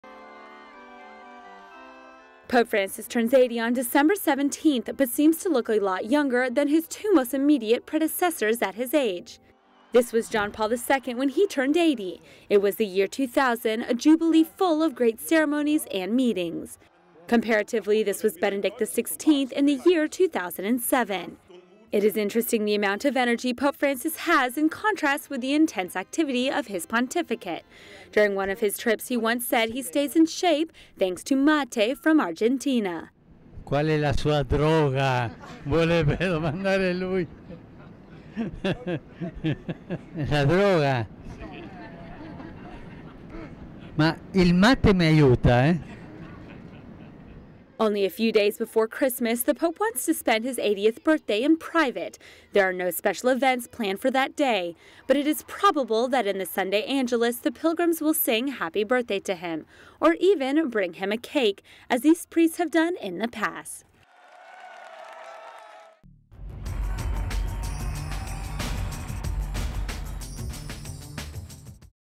The pope turns 80 without any public celebrations
Compared with his predecessors, he demonstrates great physical energy.